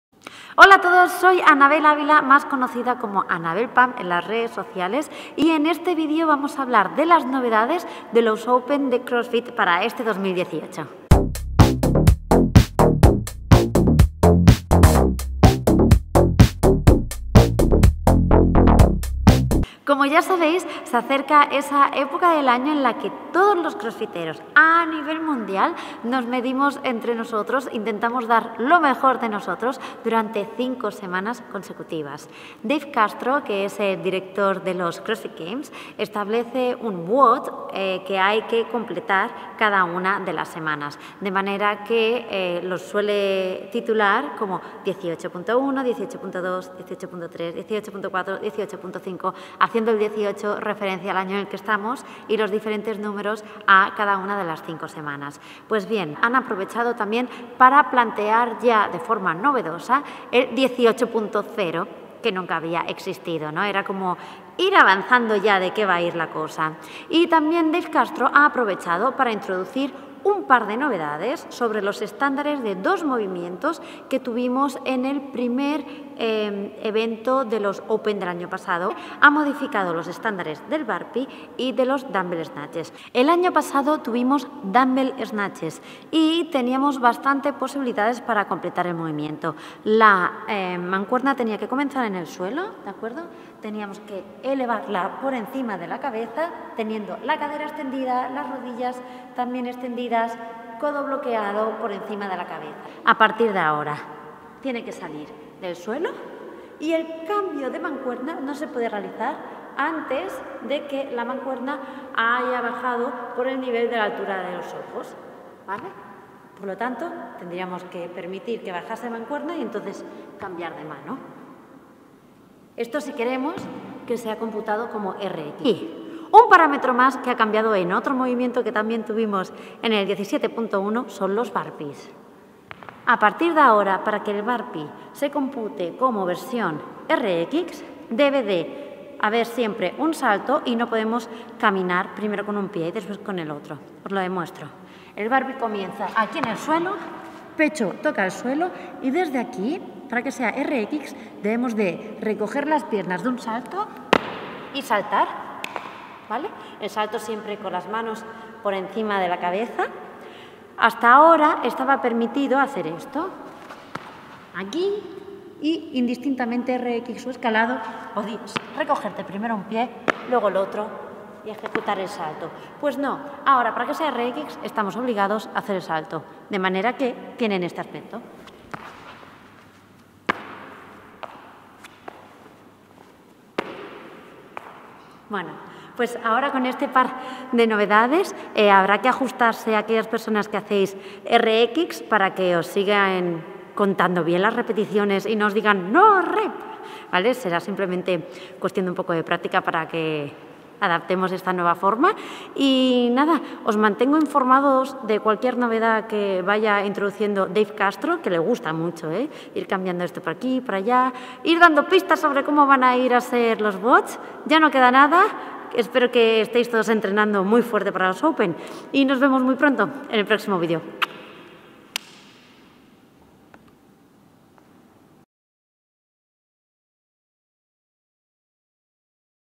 Novedades en los Crossfit Games y el 18.0
Nuestra compañera Anabel nos explica las novedades en el evento Crossfitero más relevante del 2018.

Esperemos que os guste y que nos comentéis si estáis siguiendo las indicaciones y qué beneficios estás obteniendo.